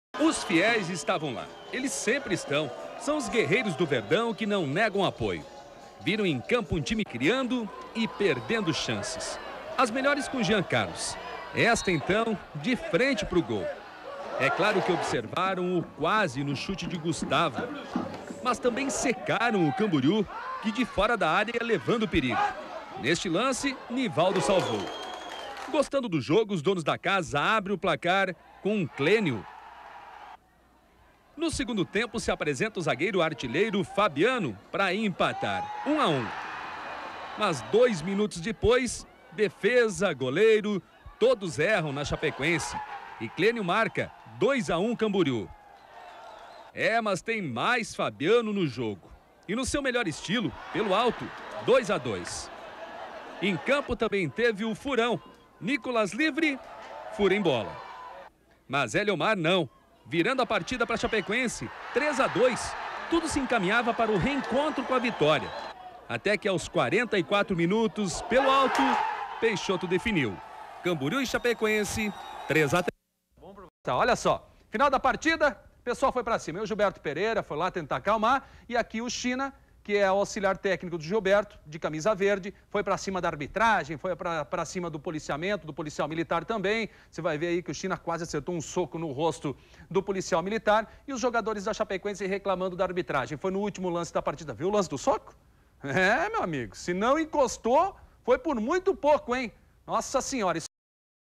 Camboriú 3x3 Chapecoense - Catarinense 2012
11/03/2012 - publico : 978
Gols : Clênio, Geninho, Peixoto (CAM)
Fabiano(2) , Eliomar (CHA)
Roberto Santos Garcia
Camboriú / SC

RBS TV